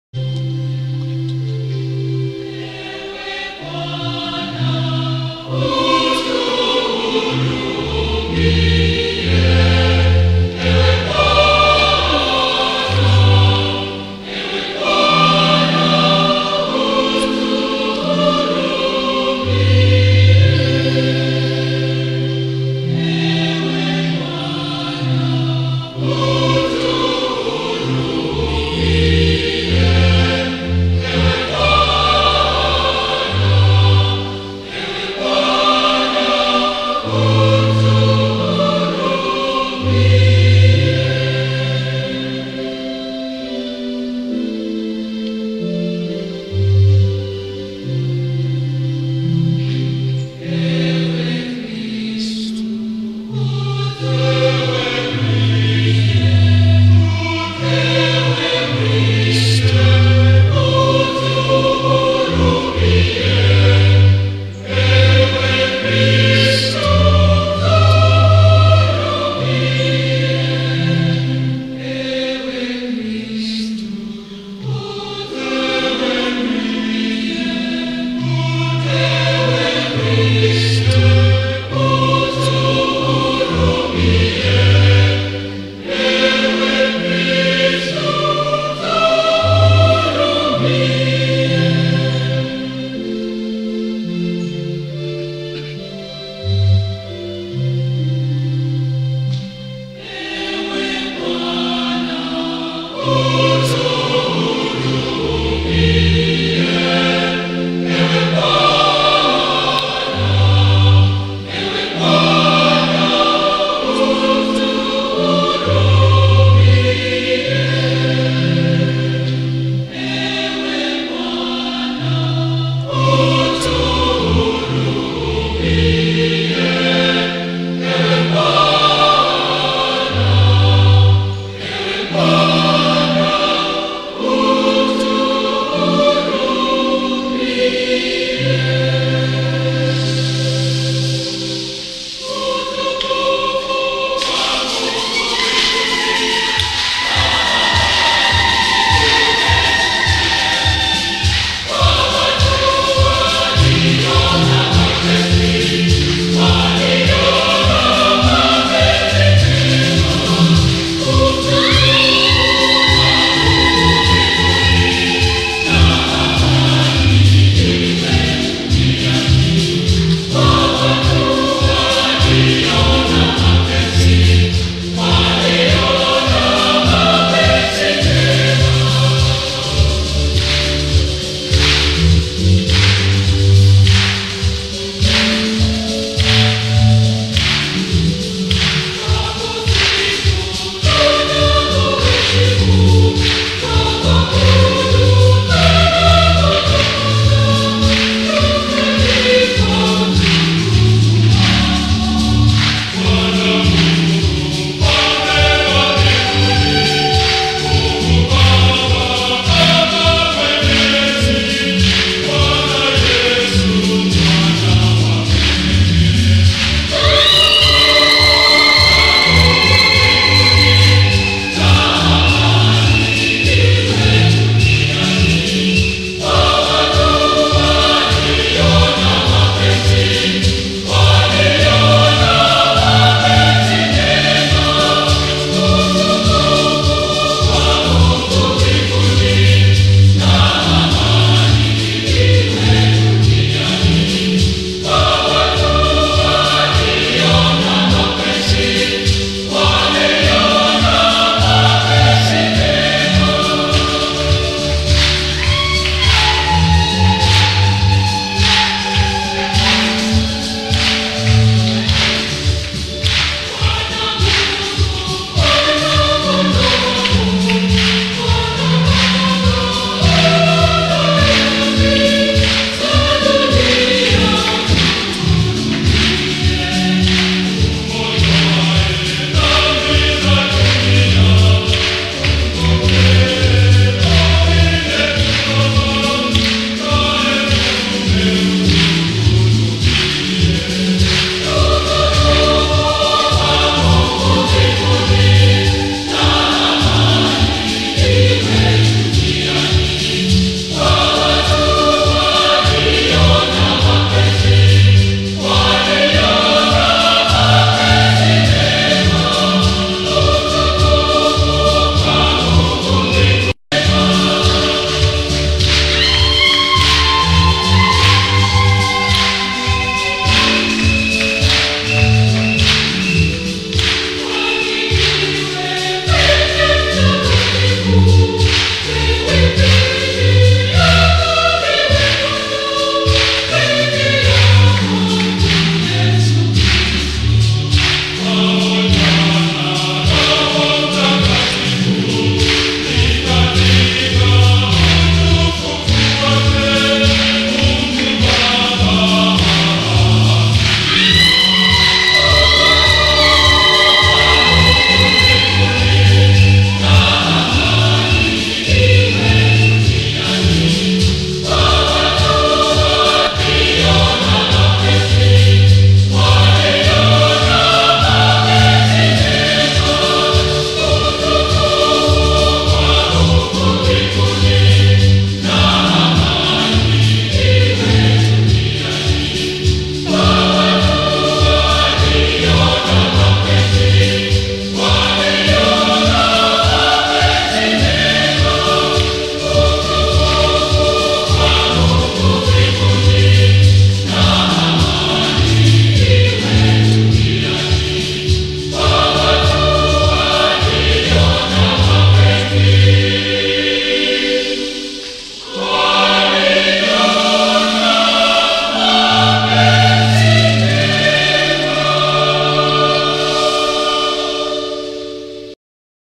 ST. MICHAEL MASS, ST. STEPHENS CHOIR KIBUYE AT VPH KISUMU PRAYER HALL LAUNCH
ST. MICHAEL MASS, ST. STEPHENS CHOIR KIBUYE KISUMU AT VPH KISUMU PRAYER HALL LAUNCH, LED BY HIS GRACE MAURICE MUHATIA